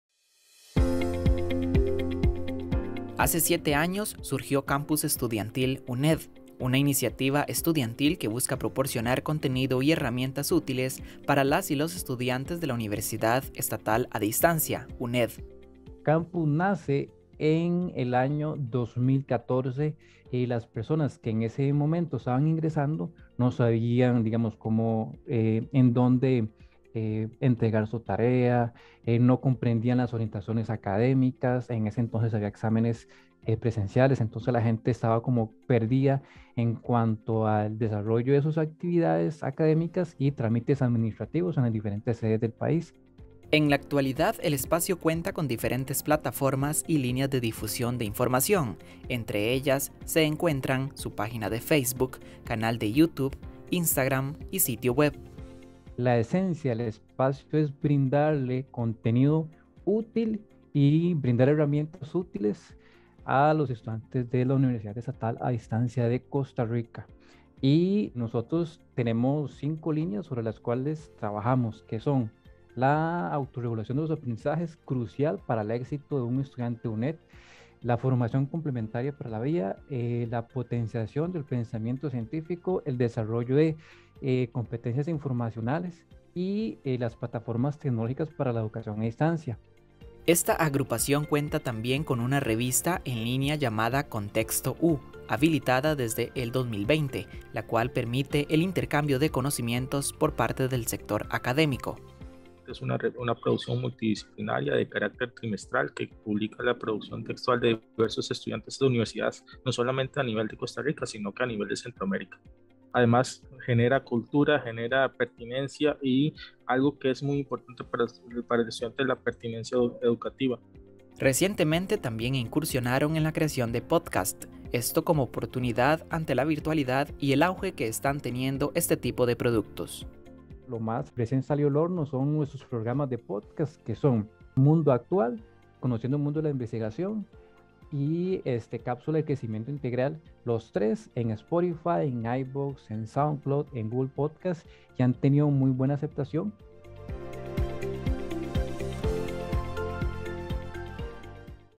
Campus Estudiantil UNED
Un grupo conformado por estudiantes universitarios y egresados de la UNED, ha desarrollado una iniciativa de acompañamiento para quienes se encuentran en la etapa universitaria. En la siguiente nota le contamos de qué se trata.